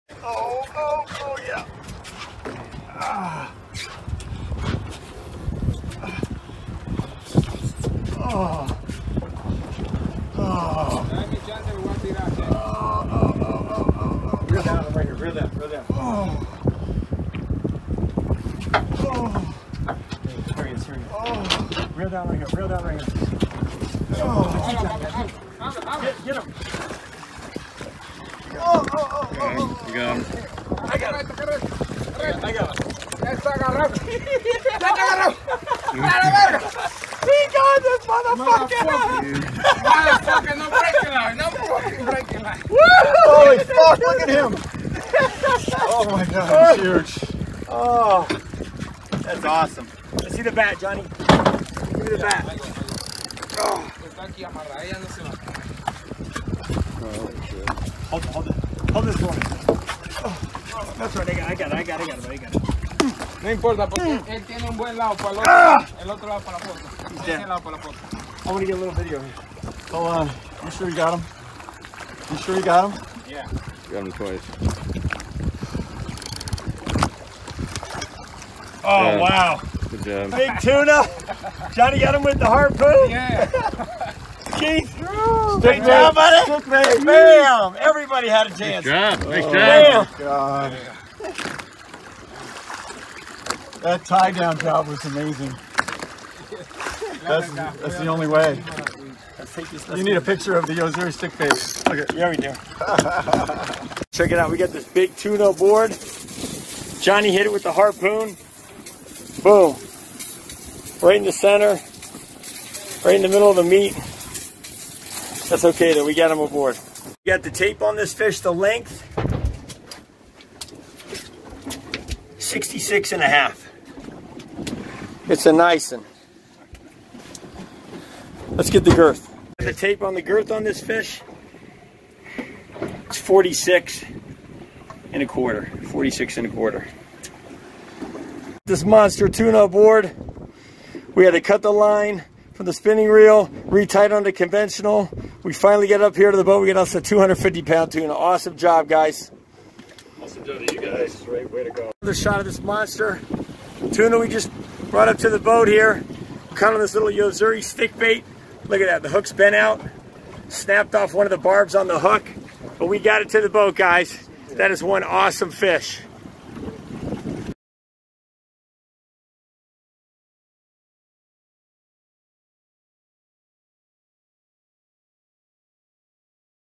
Harpoon Shot on a 250# Yellowfin Tuna in Panama!
Exciting short video of the last minute of an epic battle with a big Yellowfin Tuna!  In April 2018, our client Keith Sherin hooked this Tuna on a Yo-Zuri stick bait with a spinning rod and fought it for over two hours until we decided to take a chance and splice the braided line onto one of our Accurate ATD 30 conventional reels so we could get a bit more leverage on the fish. We were able to successfully hand line the fish for a couple of minutes while we tied the mainline onto the other reel. It took Keith another grueling 30 minutes before he got the fish close enough for my Mate Johnny to hit him with the harpoon!

Only in Panama!!!

Capt. Shane Jarvis
Sport Fish Panama Island Lodge